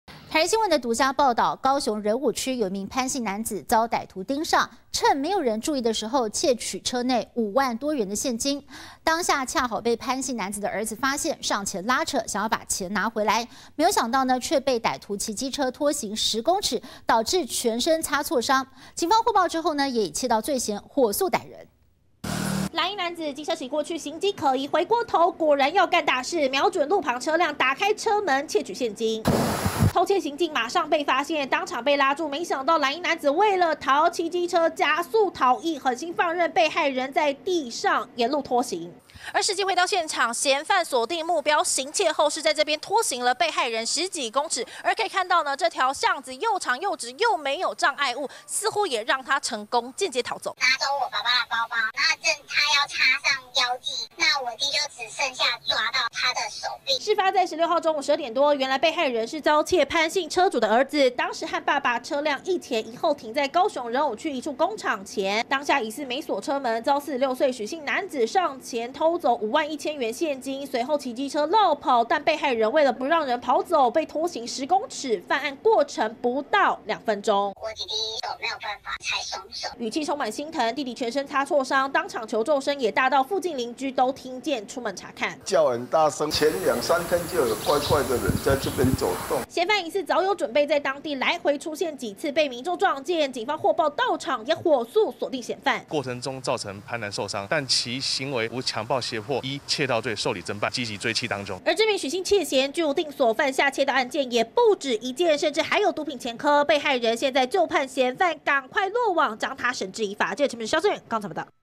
匪騎機車拖行10米 兒全身擦挫傷無大礙
台視新聞獨家報導！高雄仁武區一名潘姓男子，遭歹徒盯上，趁沒人注意，竊取車內5萬多元現金，當下恰巧被潘姓男子的兒子發現，上前拉扯，想要把錢拿回來，沒想到，卻被歹徒騎機車拖行10公尺，導致全身擦挫傷，警方獲報，也以竊盜罪嫌，火速逮人！